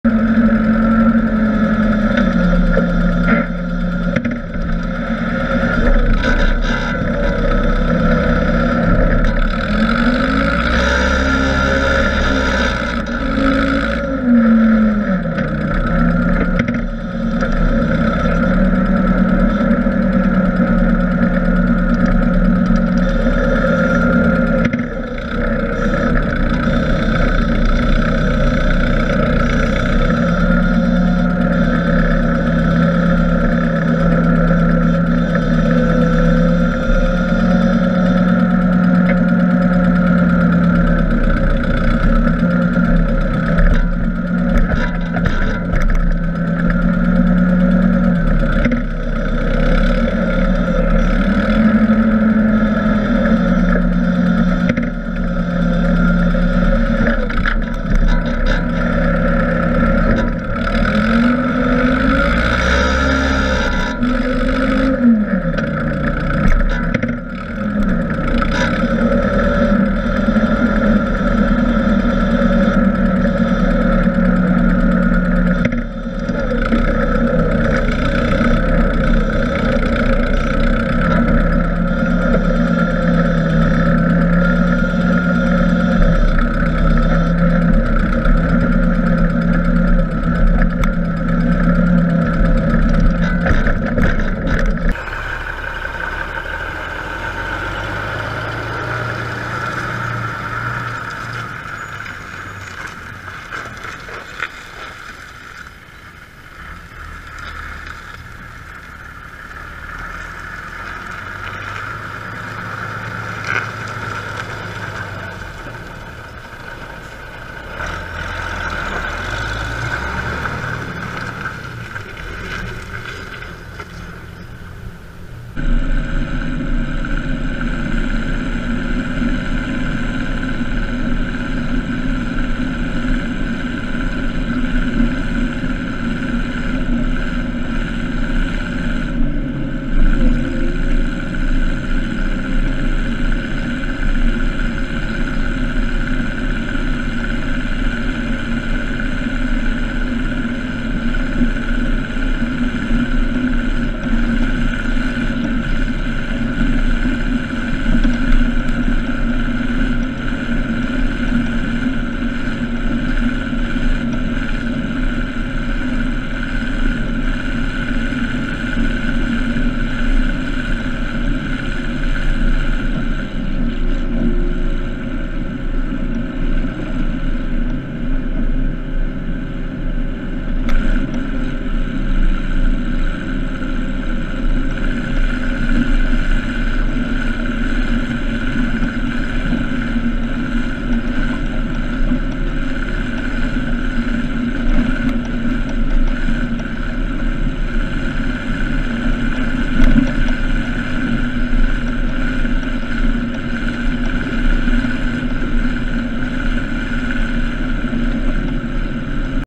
Riktigt roligt skitjobb, sprider skit 😉
Våren är här!
Vissa tycker det luktar illa , andra är beroende av det för att växa och blomstra.

Hymas 470 traktorgrävare
MB trac 1000
JF 9500 spridare (lånad)